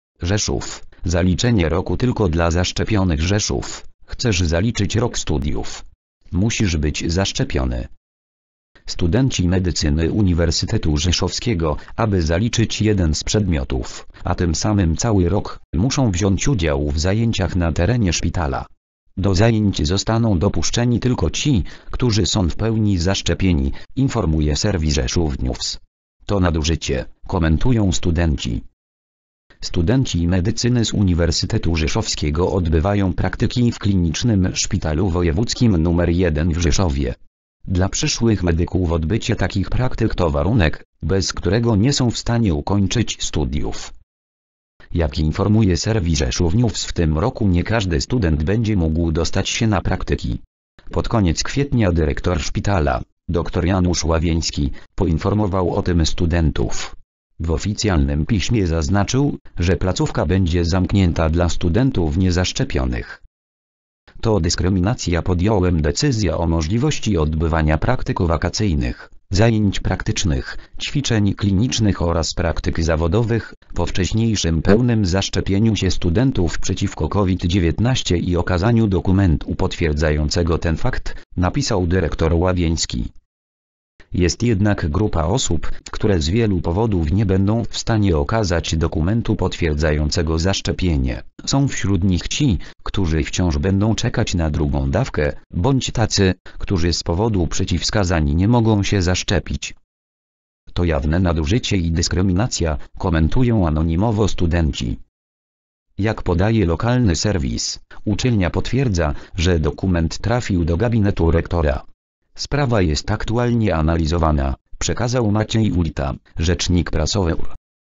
Rzeszów: Zaliczenie roku tylko dla zaszczepionych.
Rzeszów: Chcesz zaliczyć rok studiów? Musisz być zaszczepiony.

   (fot. Shutterstock/eldar nurkovic - zdjęcie ilustracyjne)